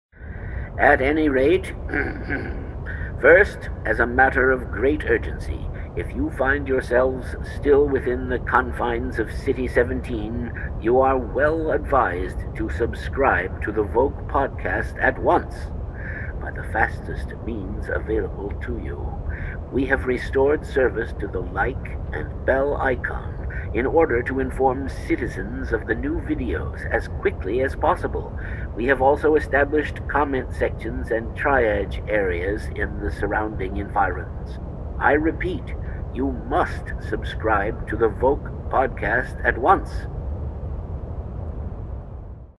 The VŌC Podcast - Dr. Kleiner to Humanity (SFM)
HAPPY HOLIDAYS!!
This is a short Source Filmmaker promo animation for The VŌC Podcast - Animated by @kajlokylo7640
Dr. Kleiner voiced by Harry S. Robins
All the models belong to Valve
Written by Ronald and Sam from VŌC
Animation by - @kajlokylo7640